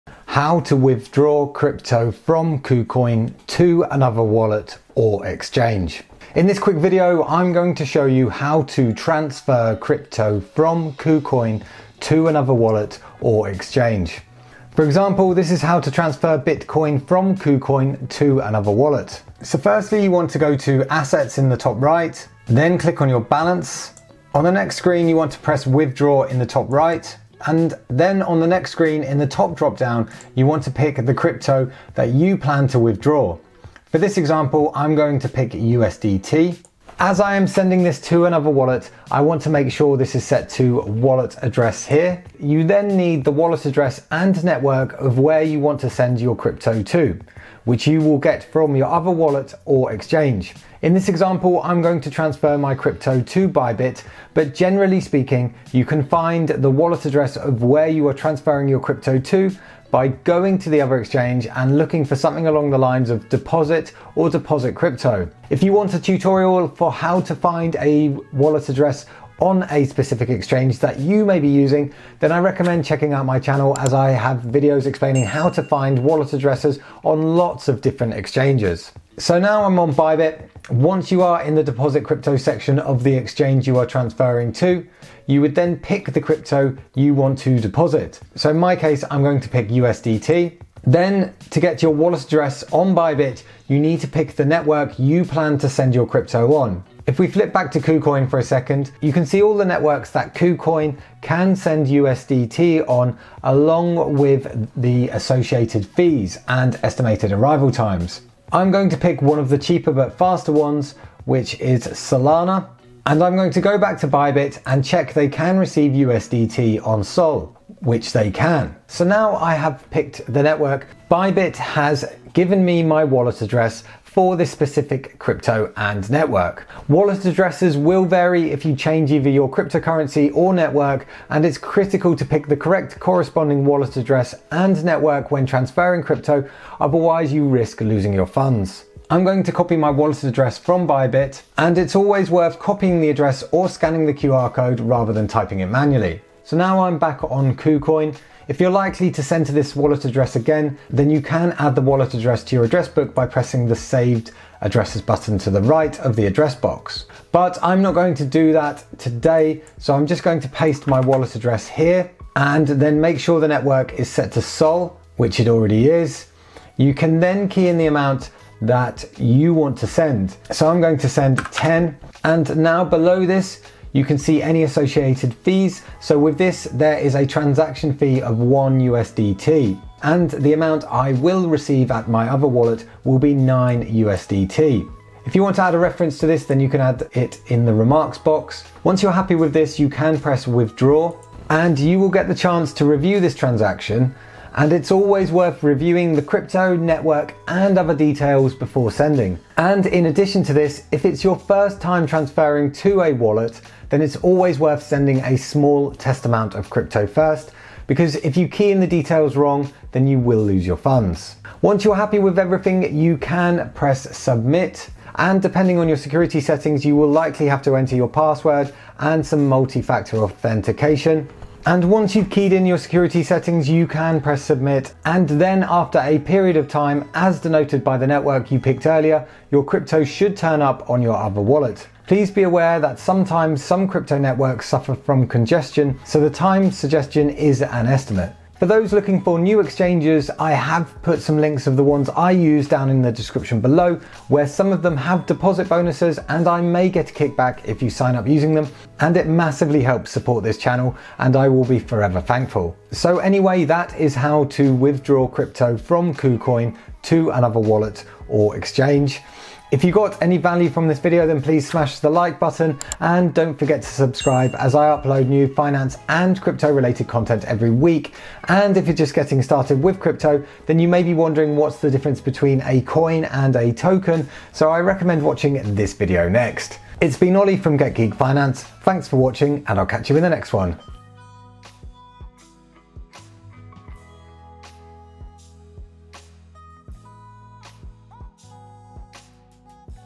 HOW TO TRANSFER CRYPTO FROM KUCOIN TO ANOTHER WALLET (2026 Tutorial)
In this quick video i will show you how to transfer crypto from Kucoin to another wallet or exchange using the Kucoin web site. Kucoin is one of the biggest cryptocurrency exchanges in the world but at some point you may want to transfer your crypto to a hardware wallet or to another exchange, so in this video i explain how to do this. In this video i show how to transfer crypto from Kucoin to Bybit.

*** CRYPTOCURENCY EXCHANGES I USE (Ad)